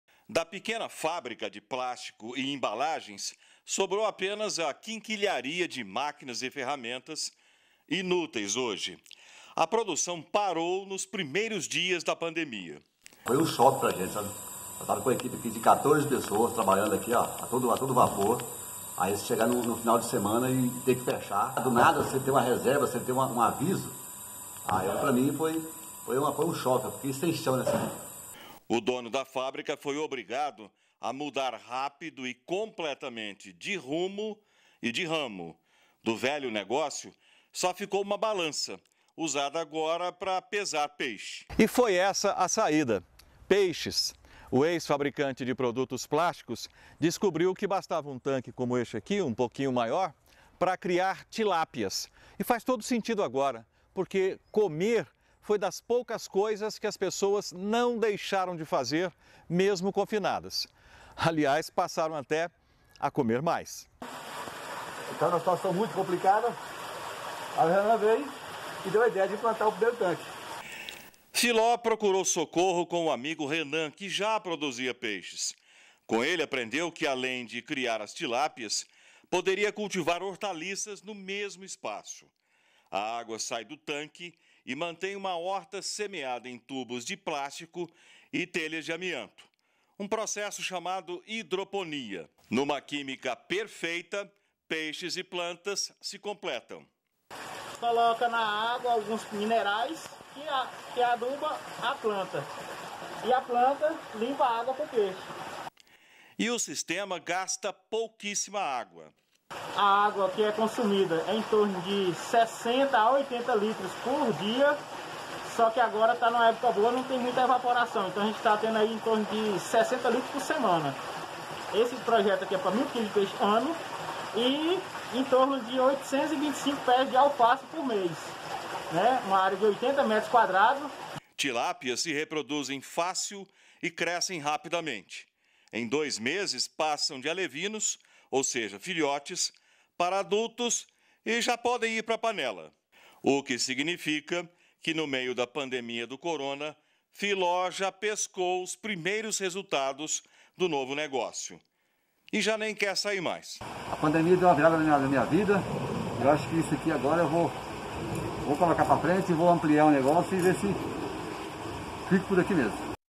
CRIAÇÃO DE PEIXES: PANDEMIA FORÇA NOVOS NEGÓCIOS
Muitos comerciantes tiveram que encerrar as atividades por causa da pandemia. Mas, boa parte não cruzou os braços não, viu. Você vai ver agora, o caso de um empresário que trocou a produção de embalagens plásticas pela criação de peixes. O novo negócio até já começou a dar lucro.